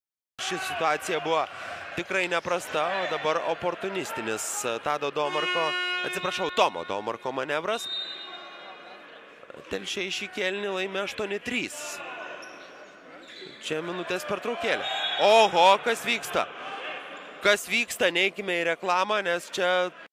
BASKET - Furieux, il met un bourre-pif à son coéquipier (Lituanie)
Osvaldas Macernis,  joueur de deuxième division lituanienne de basket, a légèrement pété un plomb lors d'un match. Mécontent de l'attitude d'un de ses coéquipiers, il s'est approché de lui pendant un temps mort et lui a collé une droite. Sympa l'ambiance.